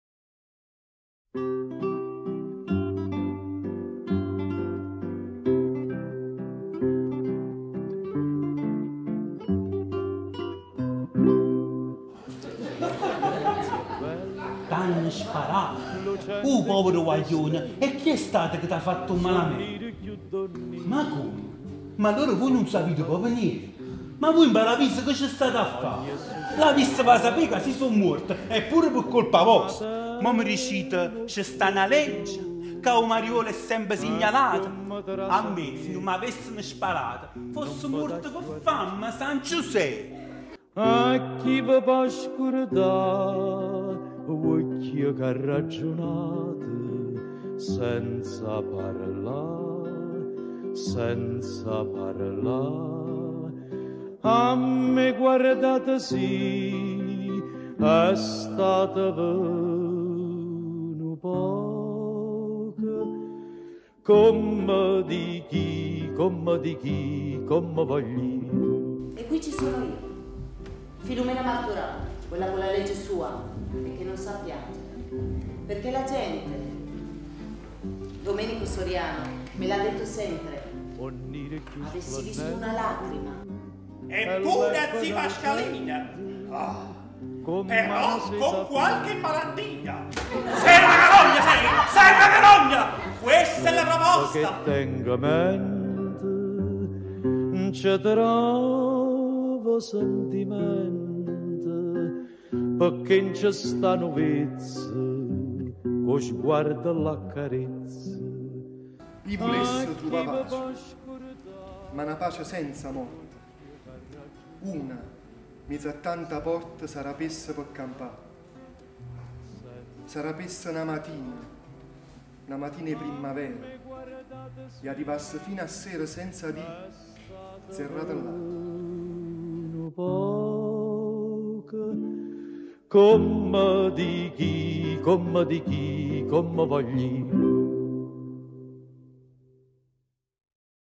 Il dono di Natale - letture al Teatro dei Segni
23 dicembre 2015

Gli attori del Teatro dei Venti e altri interpreti hanno portato in scena letture tratte dalle Commedie di Eduardo De Filippo, classici natalizi e grandi pezzi di Teatro. Palcoscenico e platea sono diventati luoghi d’incontro, focolare domestico e piazza per i migliori auguri di Natale.